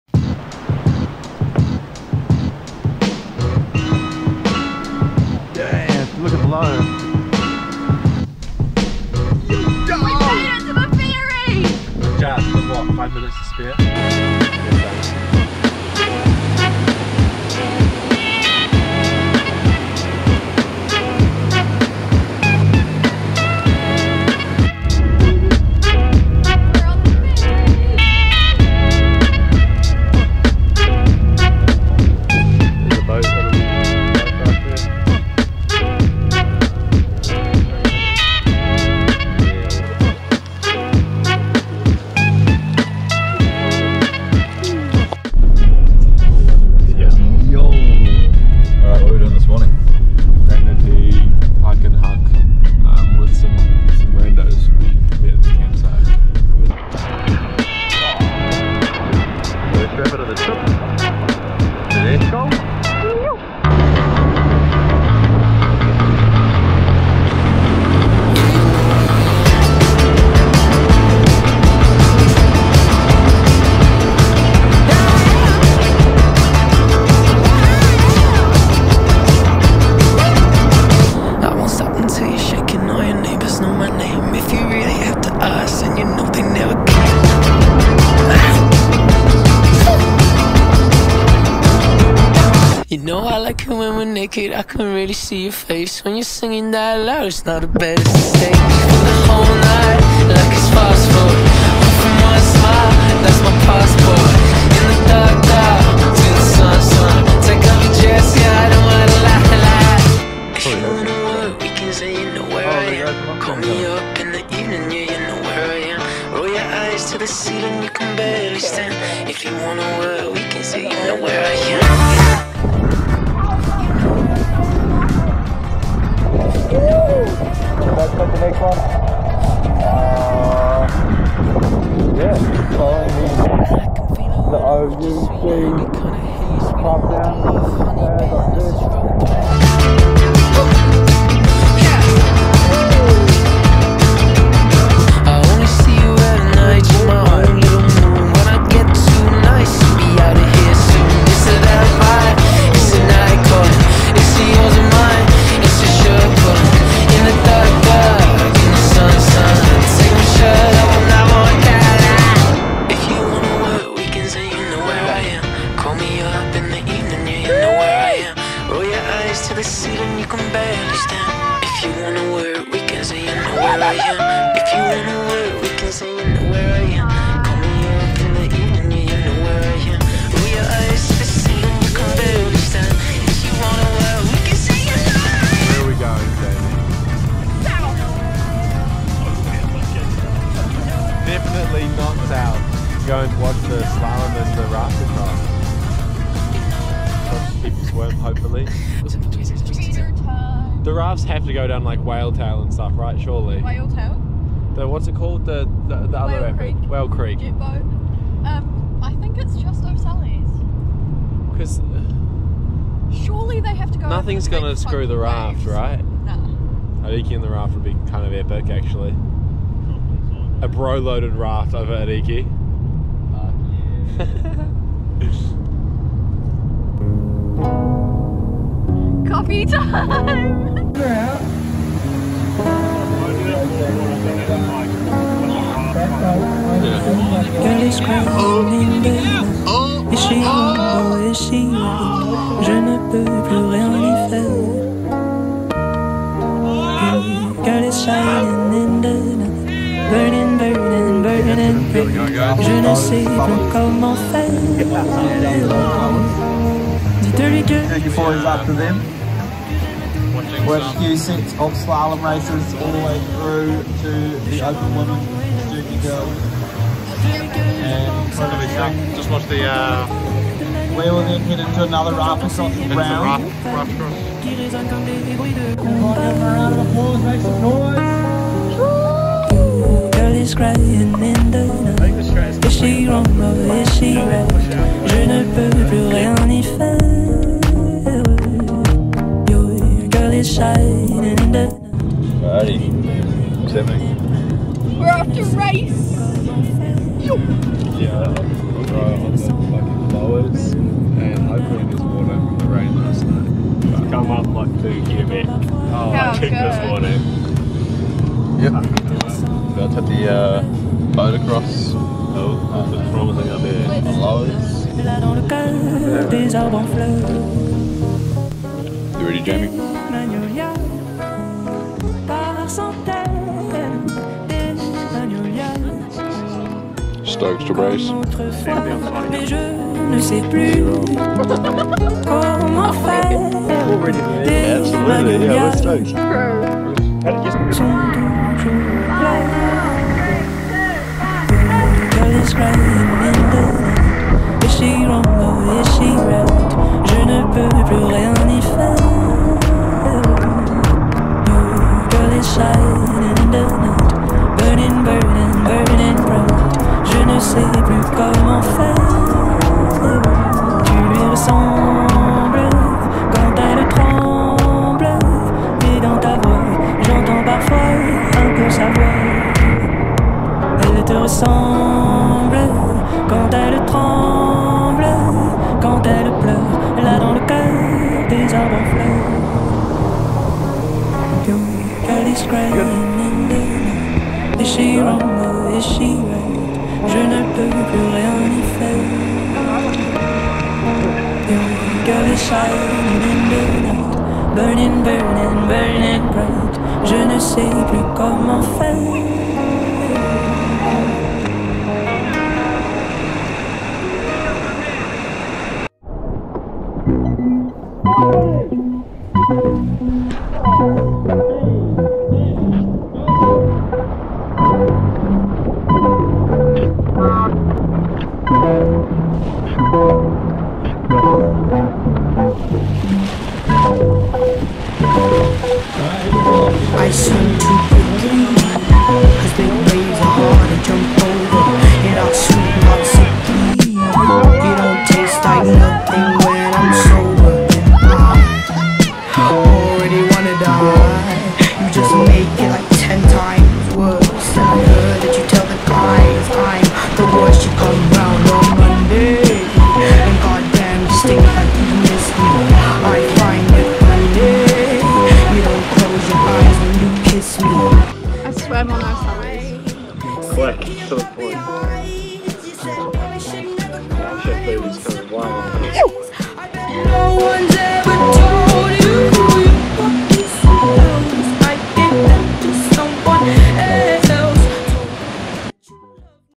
Bullerfest 2024 | New Zealand White Water Kayaking
The crew made the journey down to Murchison for Bullerfest 2024. Bullerfest is New Zealand's biggest white water festival. We traveled down on Thursday arriving Thursday night. We kayaked Granity and O'Sullivan's on Friday. I sent Ariki at the end of O'Sullivan's for the first time. Saturday was a chill day watching races and going to the festival. On Sunday morning we raced in Boater X before traveling back up to Wellington.